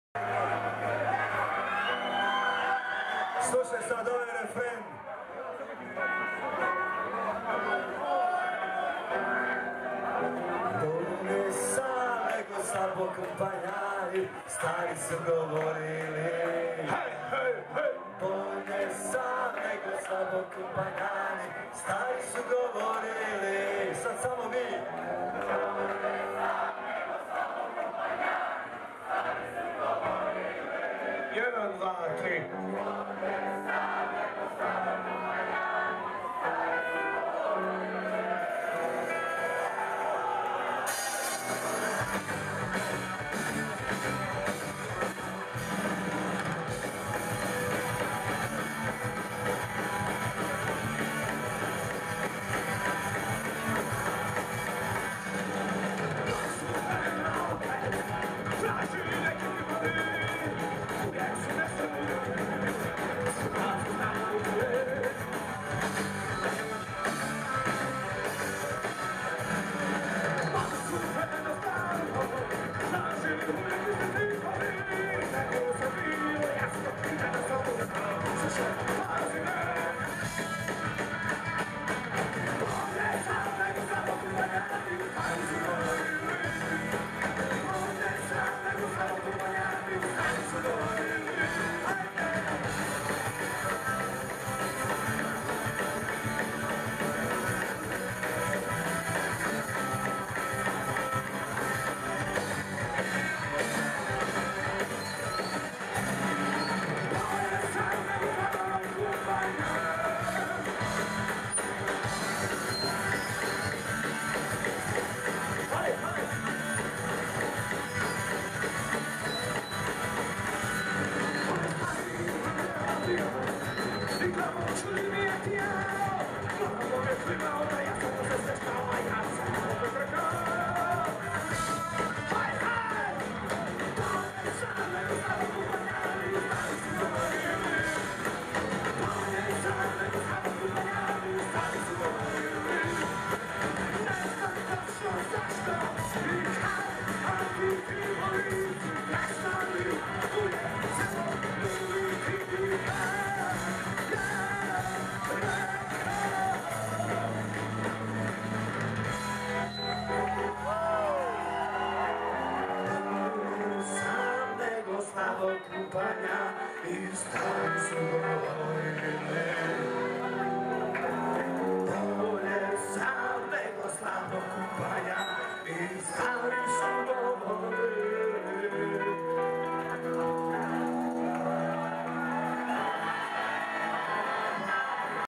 Viva La Pola! 2005 - KUD Idijoti (1)
KUD Idijoti (translation: Society of Culture and Arts "Idiots" - note that the word Idijoti is deliberately spelled wrong, the correct Croatian form of the word is idioti) is a punk-rock band from Pula, Croatia. The band was formed on February 2nd, 1981 and released their first album in 1986. In 1987 they won at the Youth Festival in Subotica and are one of the most popular bands on the punk scene of the former Yugoslavia.
Tusta - vocals
Sale Veruda - guitar, backing vocals
Dr. Fric - bass and backing vocals
Ptica - drums